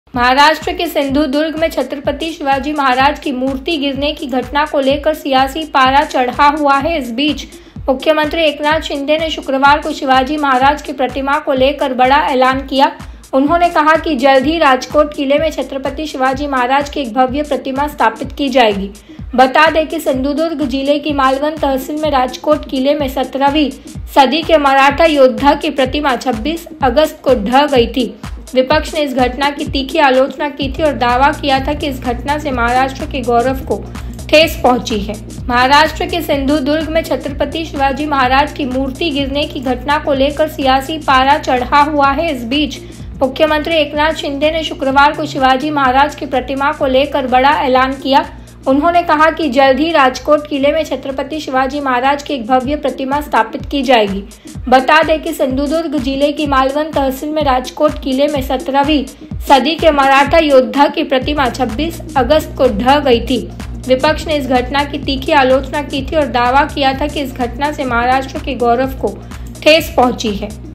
CM Eknath Shinde बोले - छत्रपति शिवाजी महाराज की एक भव्य प्रतिमा स्थापित की जाएगी
महाराष्ट्र के सिंधुदुर्ग में छत्रपति शिवाजी महाराज की मूर्ति गिरने की घटना को लेकर सियासी पारा चढ़ा हुआ है. इस बीच मुख्यमंत्री एकनाथ शिंदे (Eknath Shinde) ने शुक्रवार (30 अगस्त) को शिवाजी महाराज की प्रतिमा को लेकर बड़ा ऐलान किया है. उन्होंने कहा कि जल्द ही राजकोट किले में छत्रपति शिवाजी महाराज की एक भव्य प्रतिमा स्थापित की जाएगी.बता दें सिंधुदुर्ग जिले की मालवण तहसील में राजकोट किले में 17वीं सदी के मराठा योद्धा की प्रतिमा 26 अगस्त को ढह गई थी. विपक्ष ने इस घटना की तीखी आलोचना की थी और दावा किया था कि इस घटना से महाराष्ट्र के गौरव को ठेस पहुंची है.सीएम एकनाथ शिंदे ने कहा, "छत्रपति शिवाजी महाराज एक युगपुरुष हैं. वह राज्य और राष्ट्र के गौरव हैं. राजकोट किले की घटना दुर्भाग्यपूर्ण है. महाराष्ट्र सरकार और नौसेना द्वारा जल्द ही वहां फिर से एक भव्य प्रतिमा स्थापित की जाएगी."